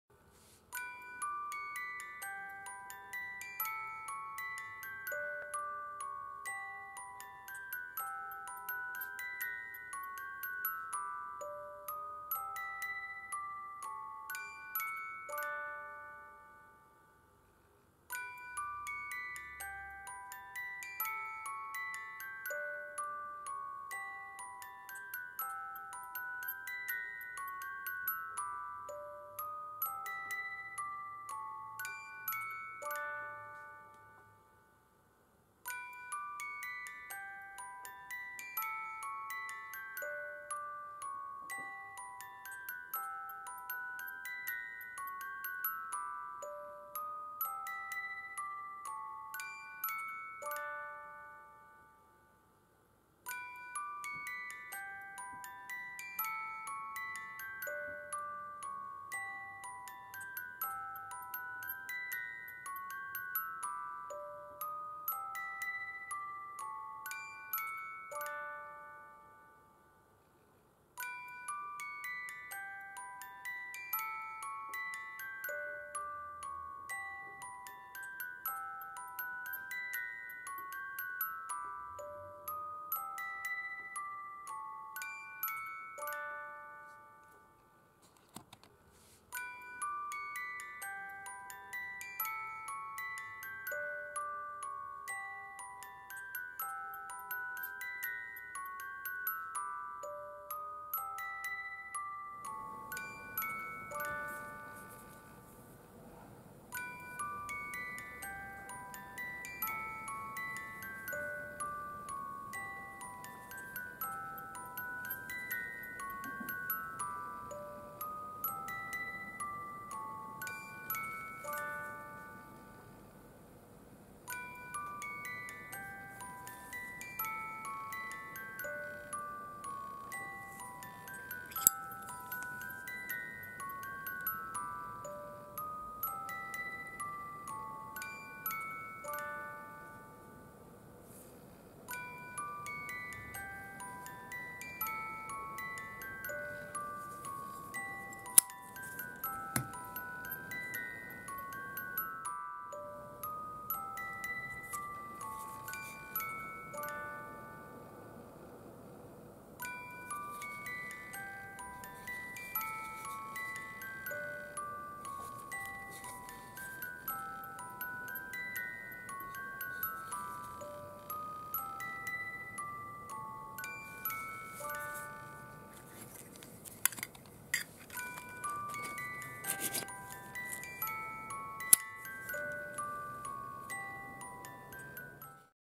[ Lighters ] Zippo #13 Slim Sterling Silver "Engine Turned"
I just wanted a sterling silver model with a italic bottom stamp.